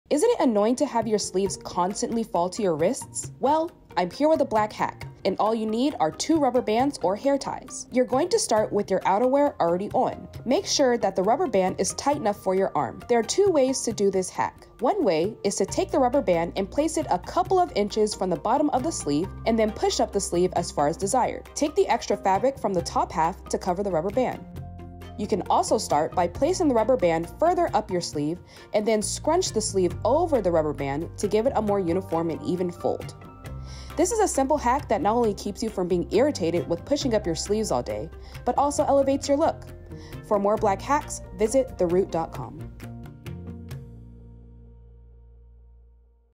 How To Keep Your Sleeves From Sliding Down | Black Hack
On this edition of Black Hack, we show you how to keep your sleeves from sliding down. This simple hack using rubber bands keeps you from being irritated with pushing up your sleeves all day.